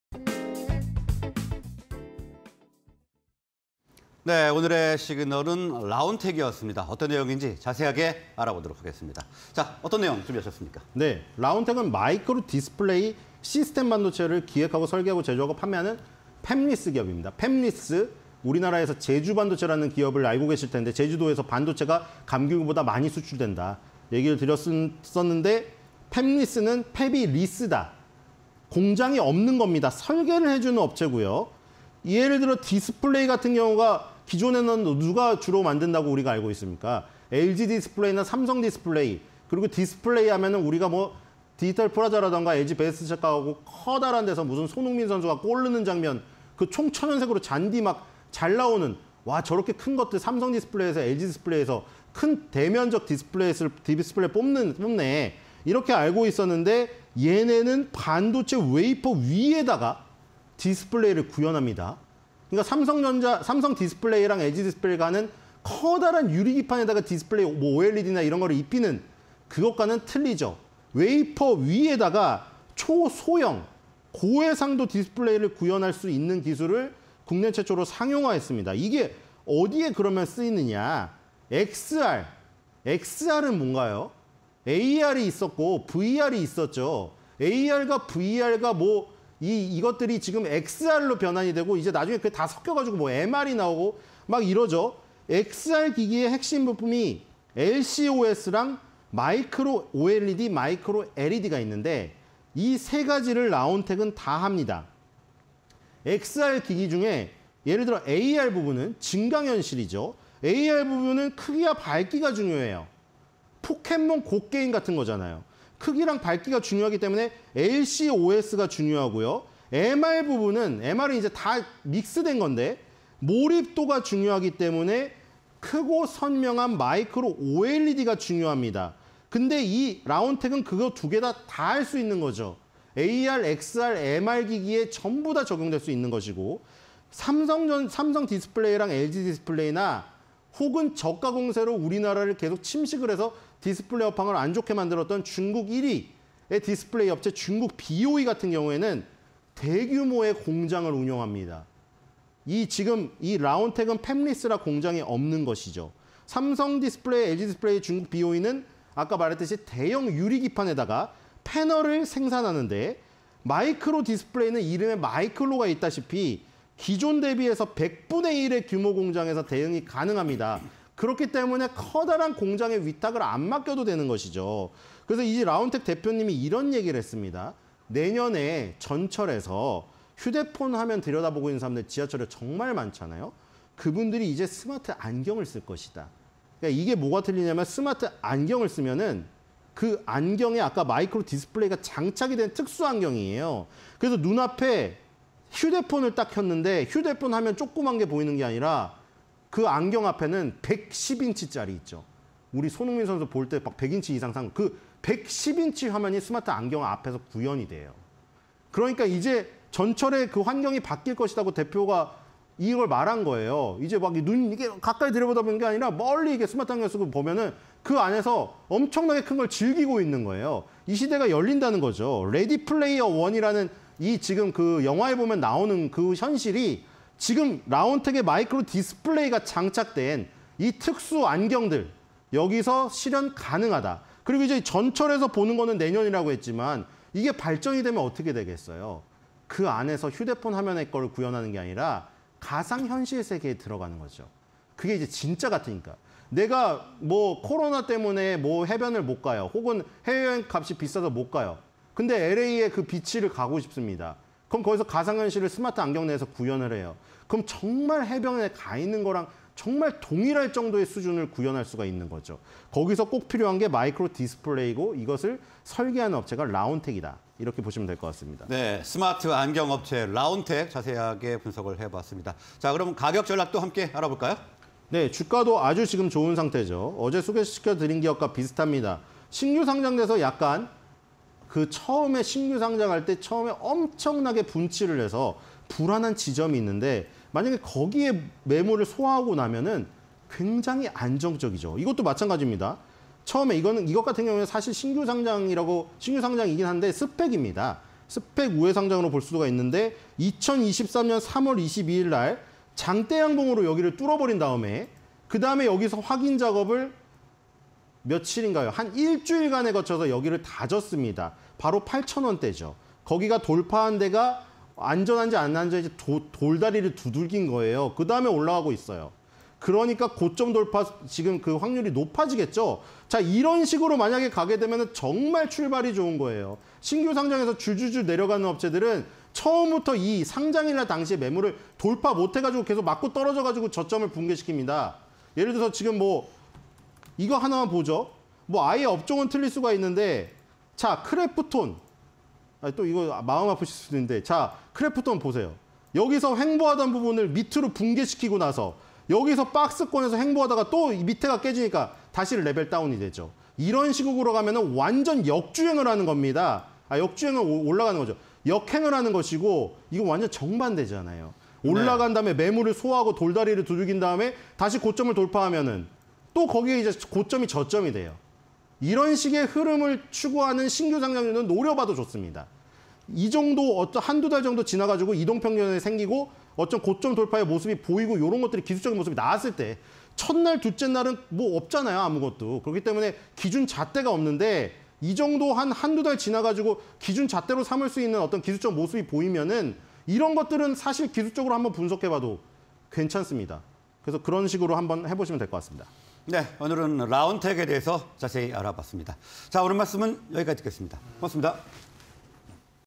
라온텍(418420), 스마트 안경 시대 온다... "레디플레이어 원 고고"_신소장의 시그널 (20230418)
라온텍(418420) "스마트 안경 시대 온다"
- 마이크로디스플레이 기획·설계·제조·판매
- 기존 대비 100분의 1규모 공장에서 대응
- 증강현실 안경 양산 개시